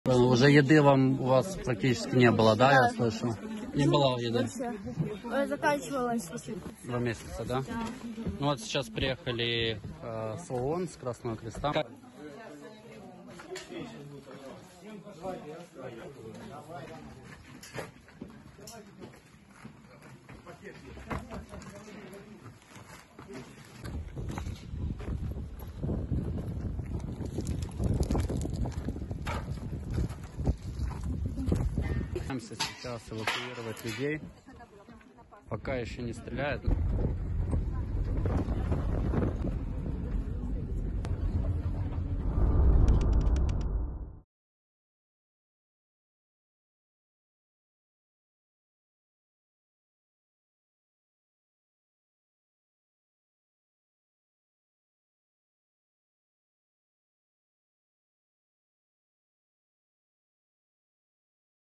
Rescatados de la última resistencia de Mariupol: "Nos estábamos quedando sin comida"
Se cree que unas 100.000 personas aún se encuentran en la sitiada ciudad de Mariupol, incluidos hasta 1.000 civiles que se refugiaron con unos 2.000 combatientes ucranianos debajo de la planta de acero de la era soviética.

Sigue la última hora de la guerra de Rusia en Ucrania con EL MUNDO